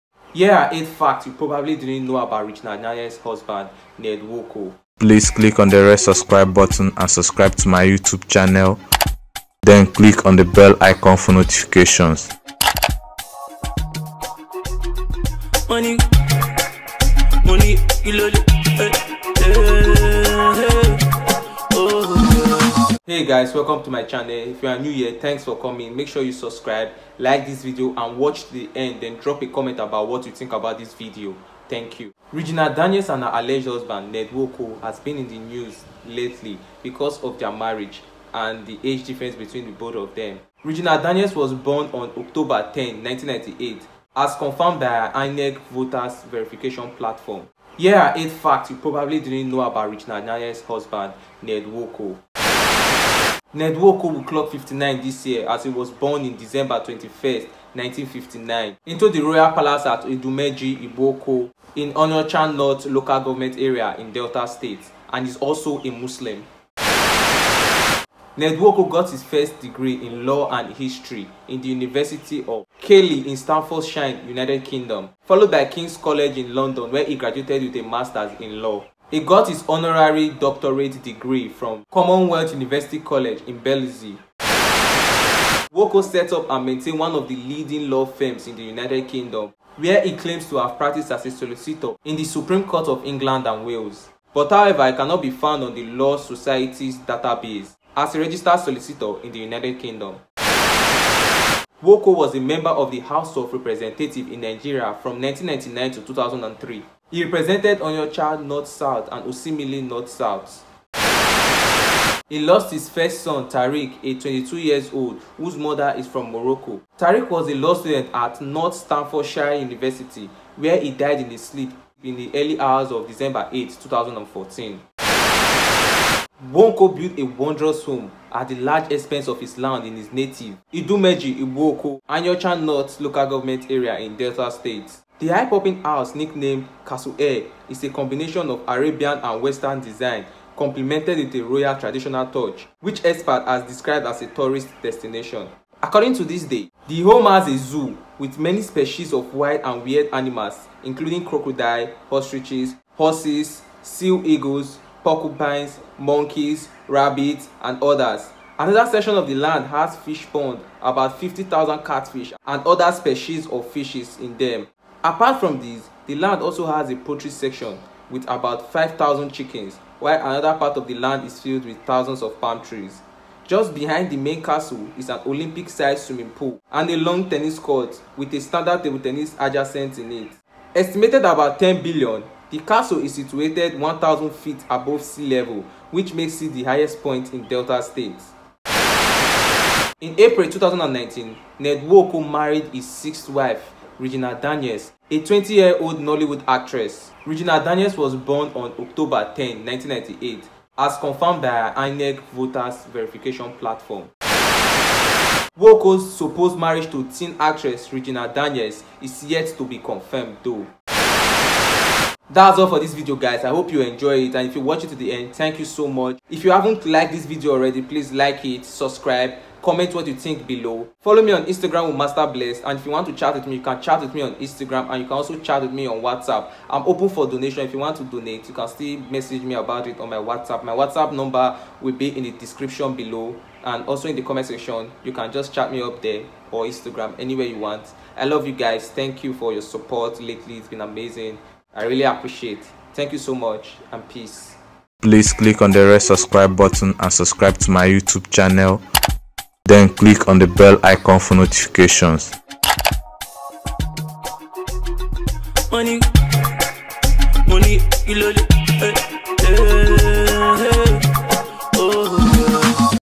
8 Facts You Probably Didn’t Know About Regina Daniels Husband Ned Nwoko
8 Facts You Probably Didn’t Know About Regina Daniels Husband Ned Nwoko